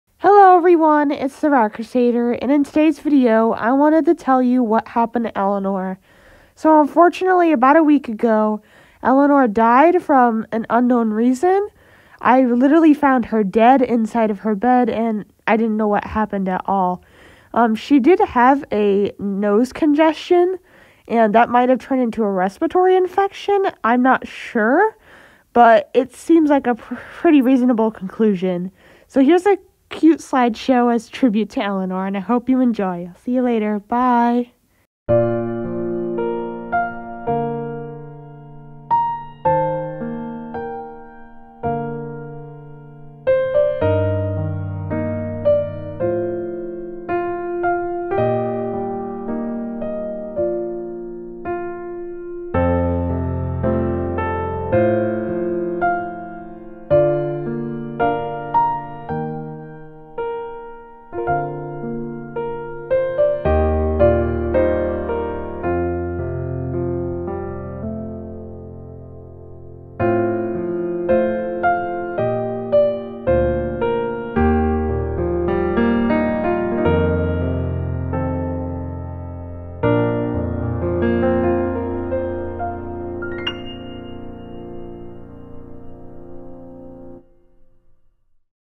😢RIP Eleanor Tribute😢
Hello everyone Eleanor died about a week ago form an unknown reason, probably from nasal congestion. Hopefully the other rats will be okay. I thank my viewers for all of their support and thank my sweet little girl for the fun we had together. 😢